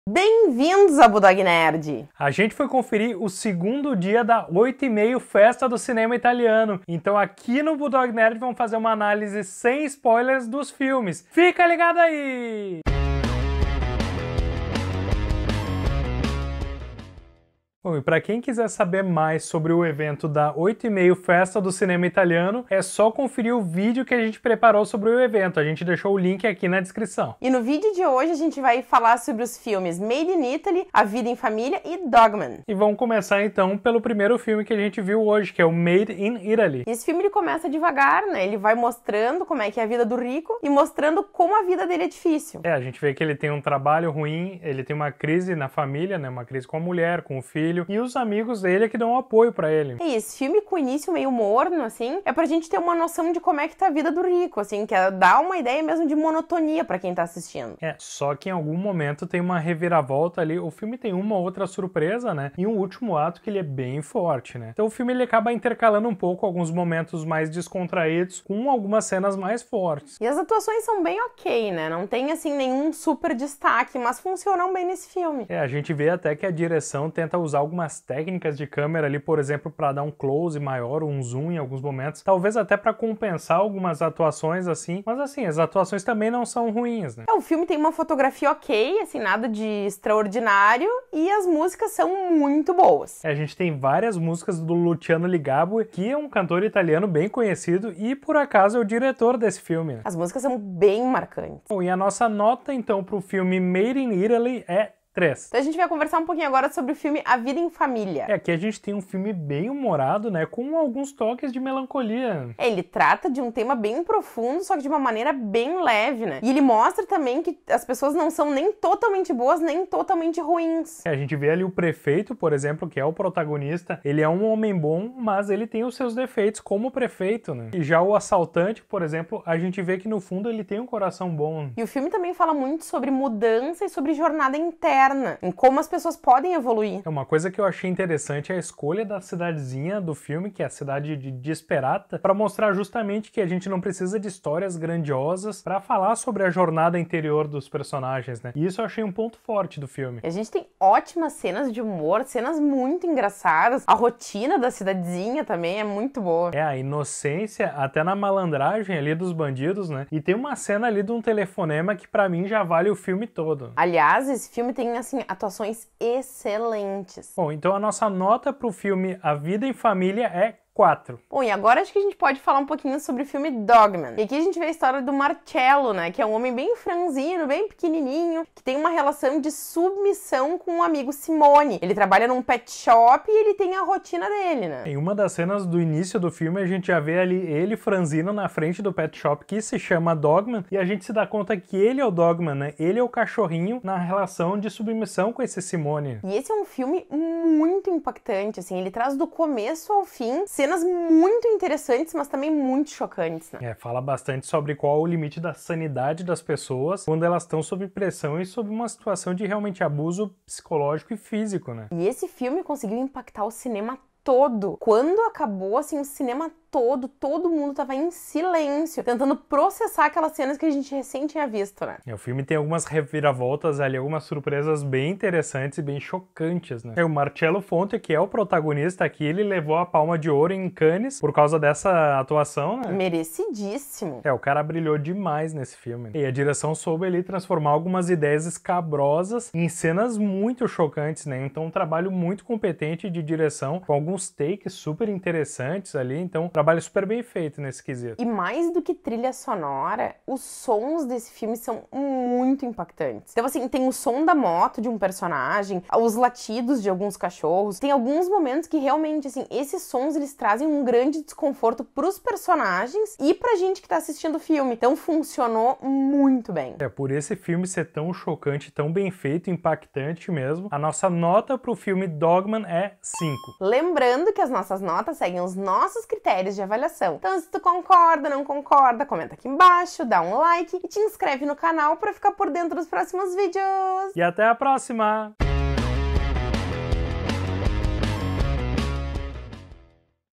8 ½ FESTA DO CINEMA ITALIANO 2018 - parte 2 (Made in Italy / A Vida em Família / Dogman)
Análise sem spoilers dos filmes "Made in Italy", "A Vida em Família" e "Dogman", que integram a 8 ½ Festa do Cinema Italiano.